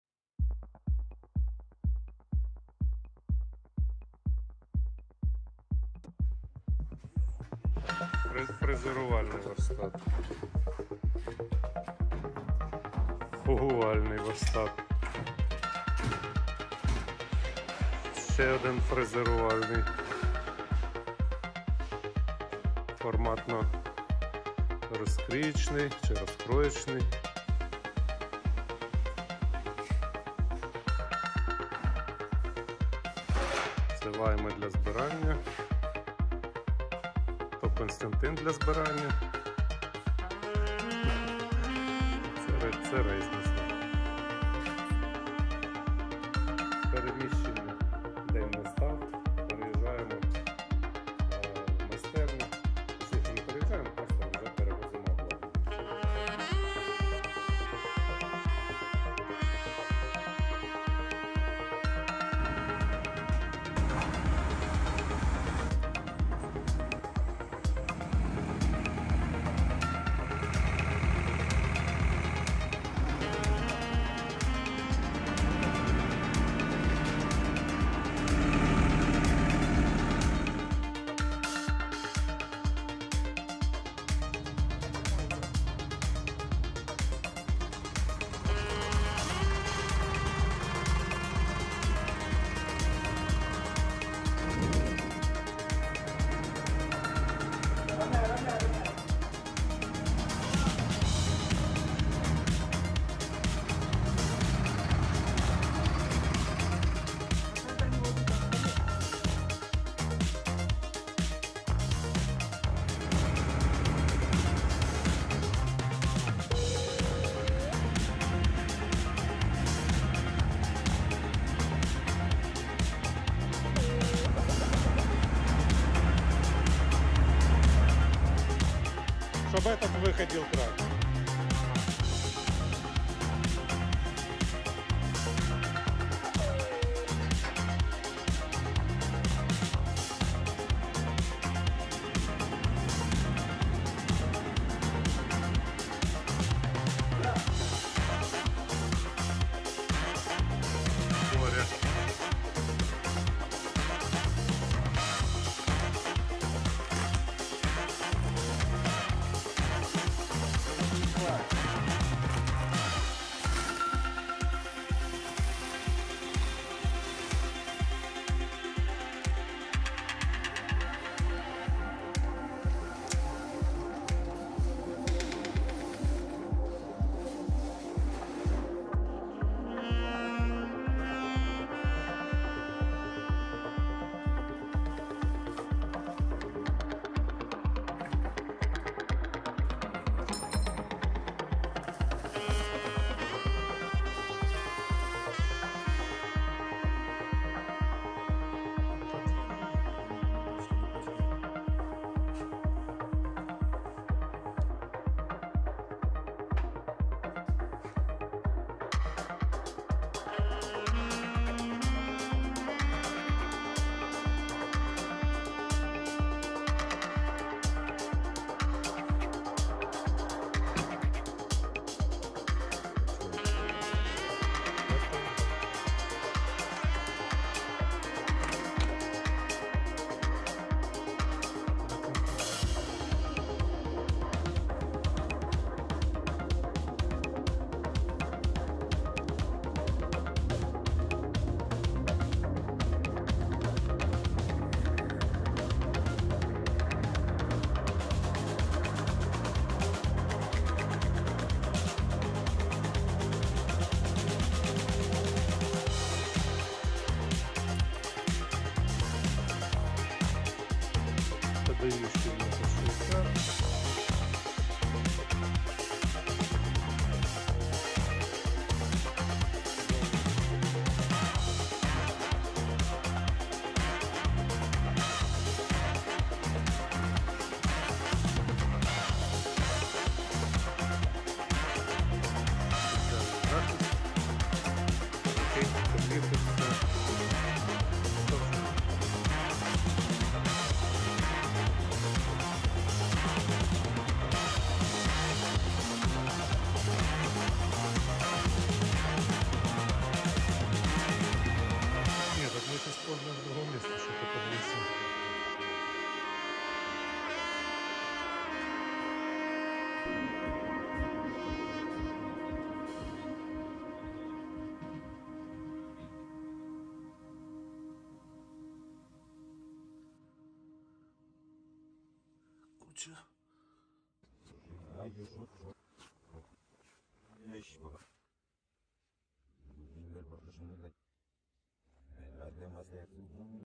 Нове обладнання для майстерні – новий творчий етап.
Багато хто знайомий із моїми виробами, знає рівень, якість та підходи до виконання. На жаль, реалізовувати нові проекти на доісторичному обладнанні стає дуже складно. Давно назріла потреба в оновленні обладнання.
Цей день настав, отже нова майстерня «Wooden Studio»!
Далі буде 🇺🇦